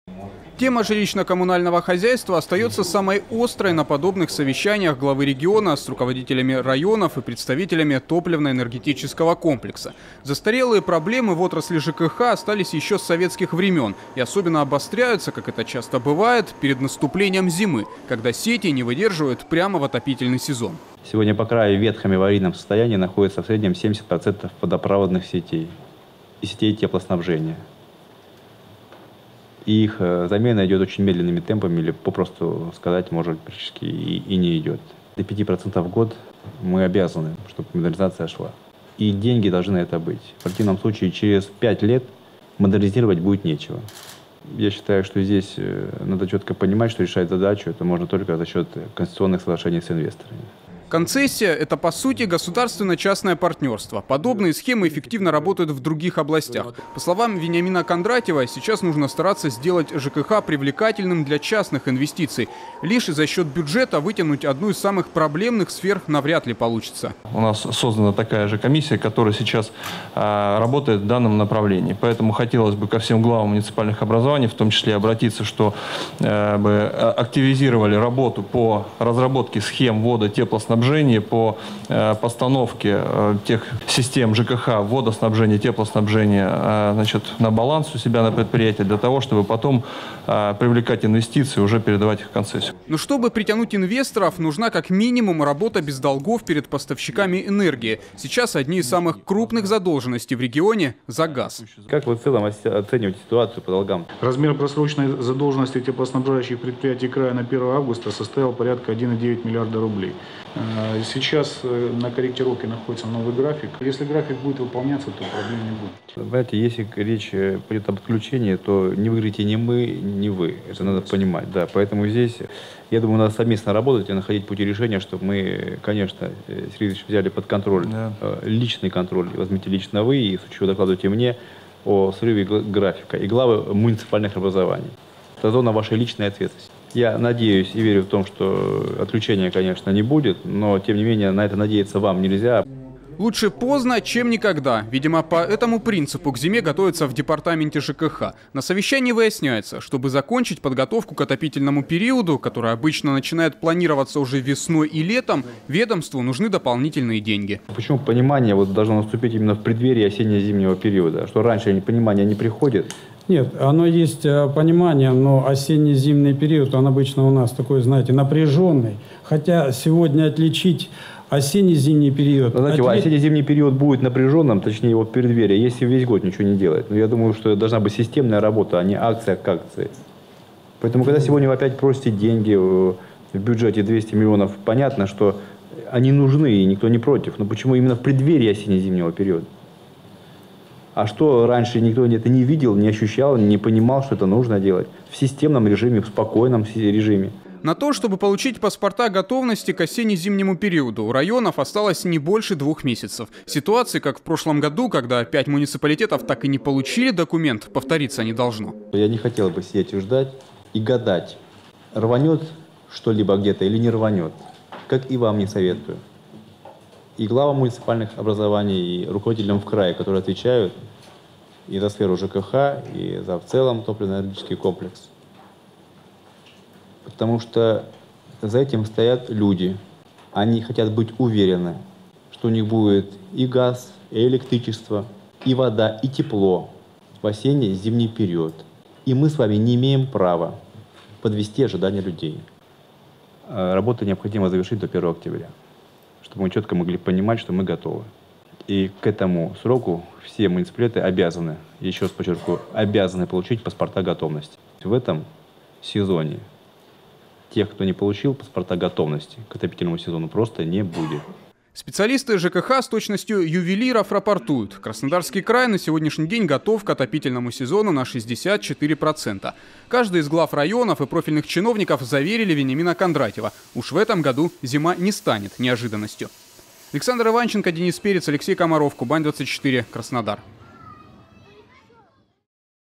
Глава региона провел селектор по подготовке к зиме. Новости Сочи Эфкате
Если не начать сейчас, через пять лет модернизировать будет нечего. Так,
коротко и понятно, Вениамин Кондратьев сегодня описал положение дел в
краевом ЖКХ. В частности речь шла о состоянии сетей водо- и
теплоснабжения. Чтобы исправить ситуацию, нужны частные инвестиции. Этот
вопрос неразрывно идет и с подготовкой к осенне-зимнему периоду, которая
стала основной темой совещания.
Сочи. Новости.
Телеканал Эфкате.